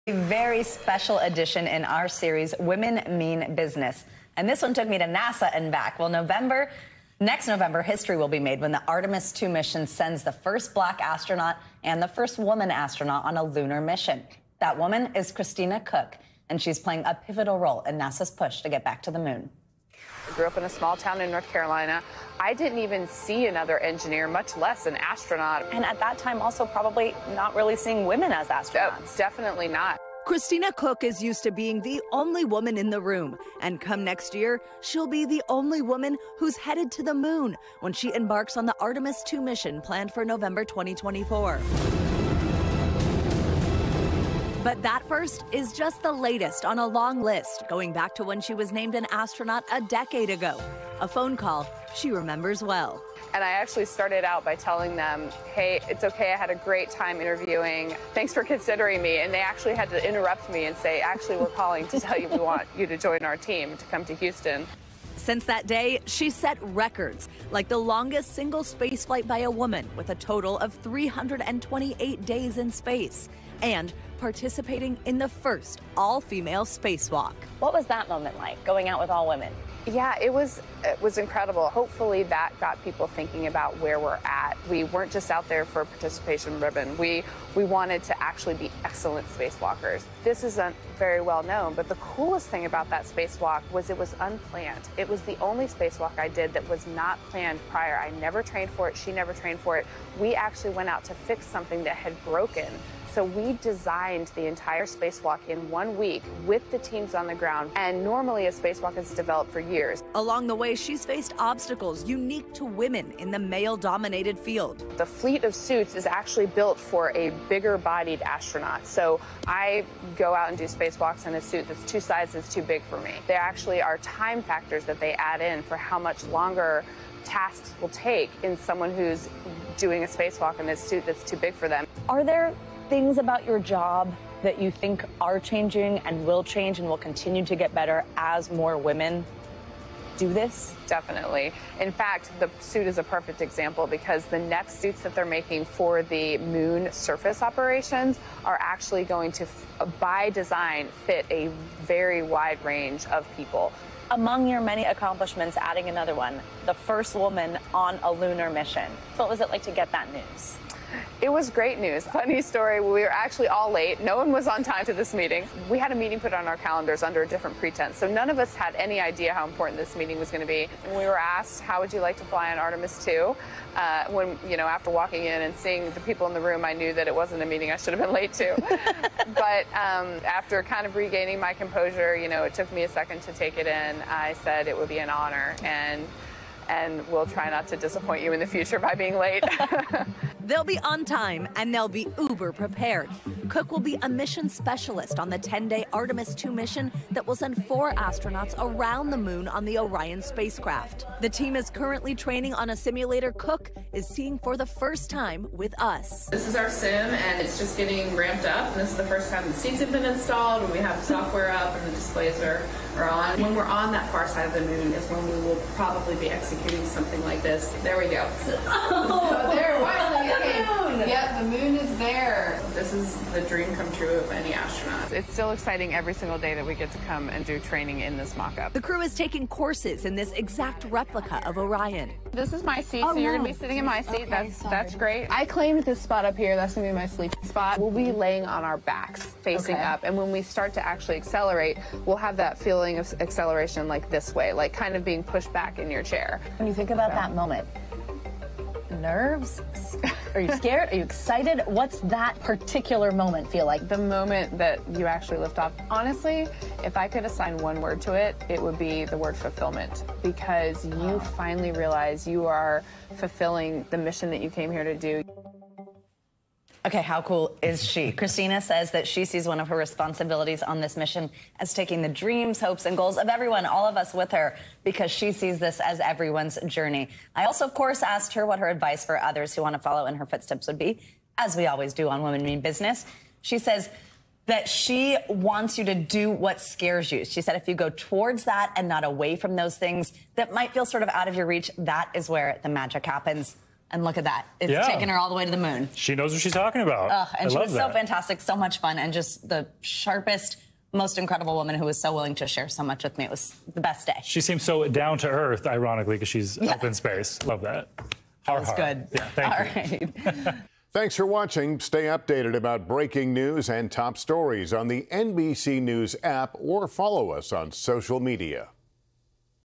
Trailblazing NASA astronaut will be first woman on Lunar mission
NBC News’ Savannah Sellers interviews Christina Koch, an astronaut used to being the only woman in the room. Koch is going on a lunar mission, the Artemis 2, in November 2024. Facing obstacles in a male dominated field, Koch explains a mission no woman has done before, to the moon and back.